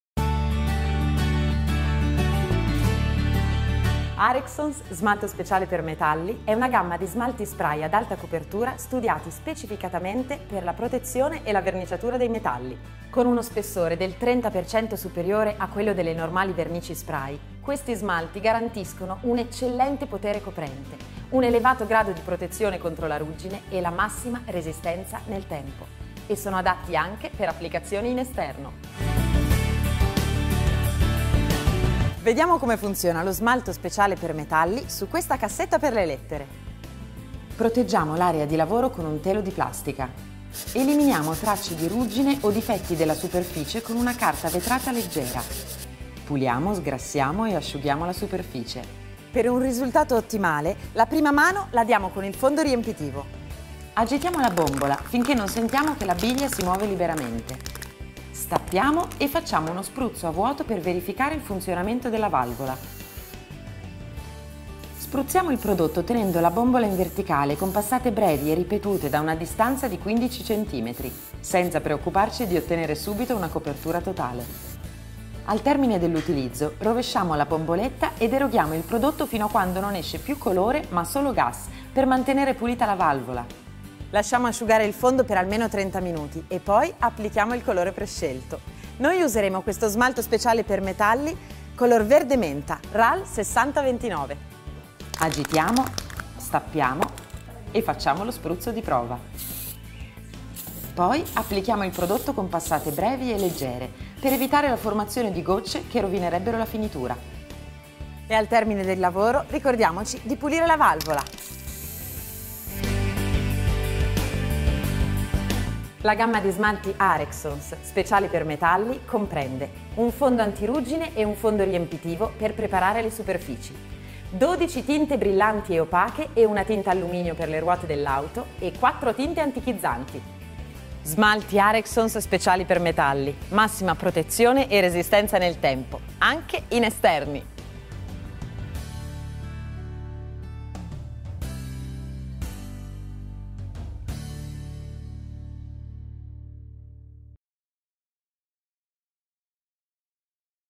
Smalti spray per verniciare il metallo
Arexons smalto speciale per metalli è una gamma di smalti spray ad alta copertura studiati specificatamente per la protezione e la verniciatura dei metalli.  Con uno spessore del 30% superiore rispetto a quello delle normali vernici spray, questi smalti garantiscono un eccellente potere coprente, un elevato grado di protezione dalla ruggine e la massima resistenza nel tempo, e sono adatti anche per applicazioni in esterni. Vediamo come funziona lo smalto speciale per metalli su questa sedia da giardino.